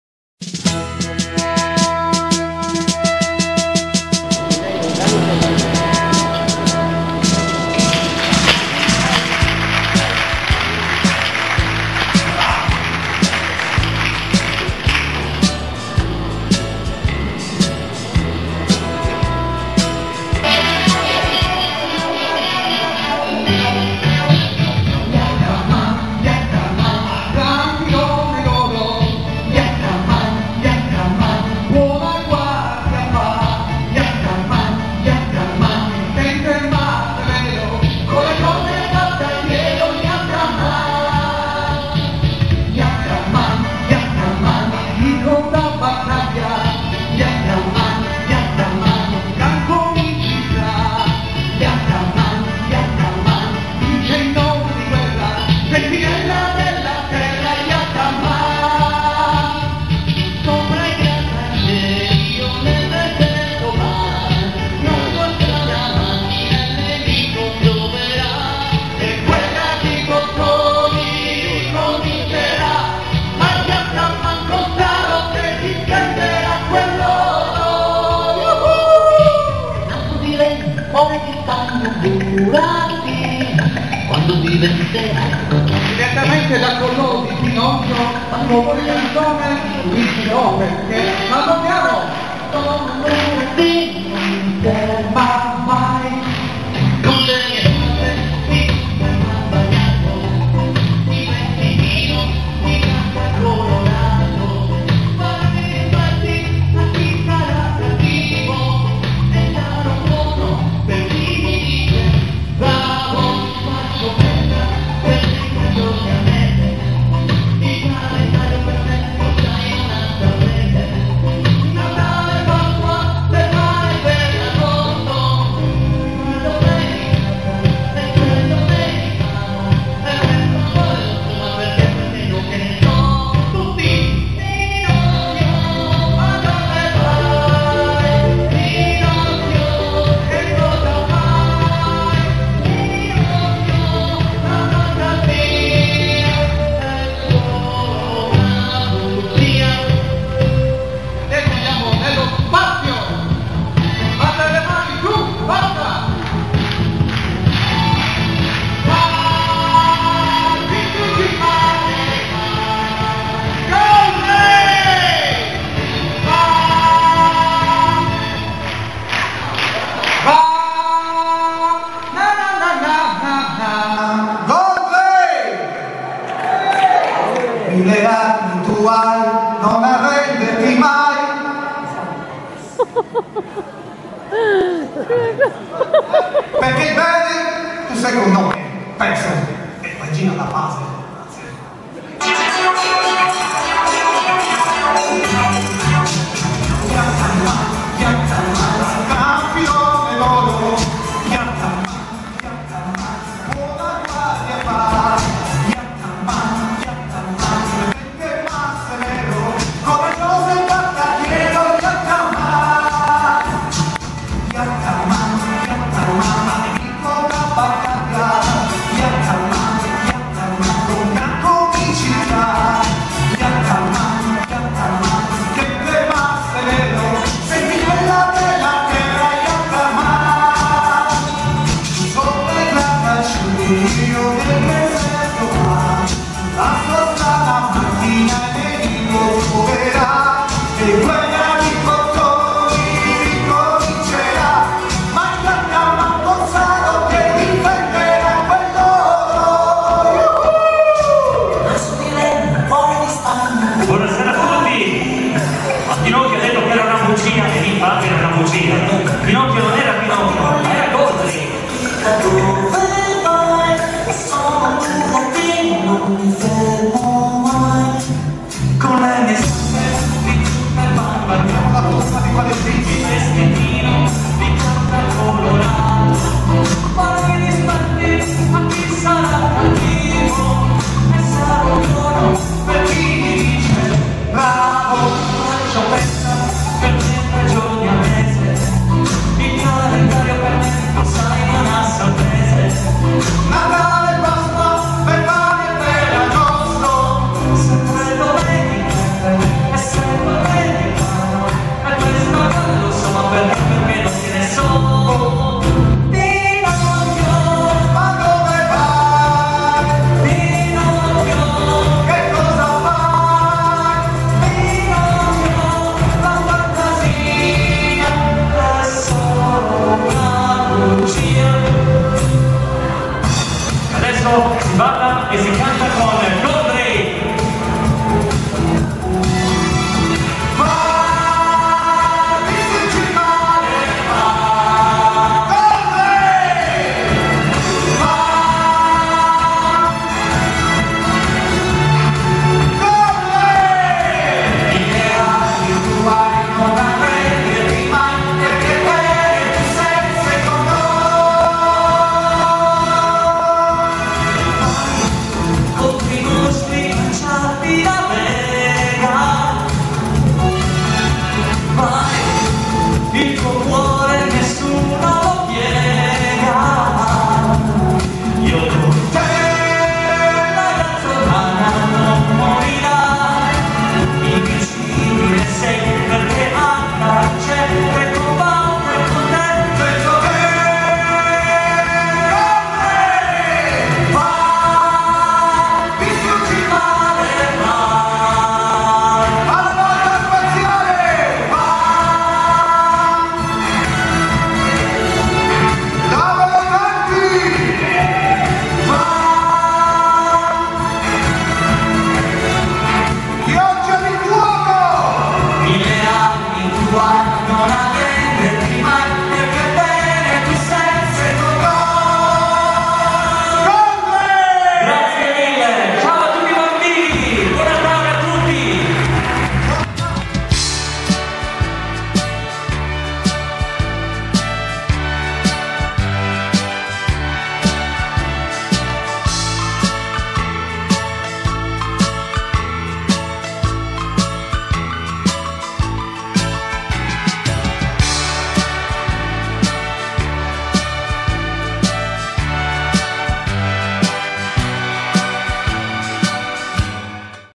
Rockin Hive al Teatro Massimo Cagliari
Esibizione dei "Rockin Hive" al teatro  "Massimo" di Cagliari in occasione della serata di beneficenza organizzata dalla croce rossa italiana il 20/12/2012 sul palco

Giovanni Jack Ardito ( Jack Sparrow )
Giuseppe Joey Angioy  ( Pinocchio/Actarus )
Luca Elvis Corvetto ( Chitarra )
Roberta Monteverde ( Prima Ballerina )
Eleonora Pippia ( Seconda ballerina )
Riprese Sami ( Dj Satomi) e Massimo Veglio
Un ringraziamento a Krystian Zeta ( Mimo Renato Zero ) per averci invitato
ai presentatori  Lello De Luca, Giovanna  Dessena
e la Croce Rossa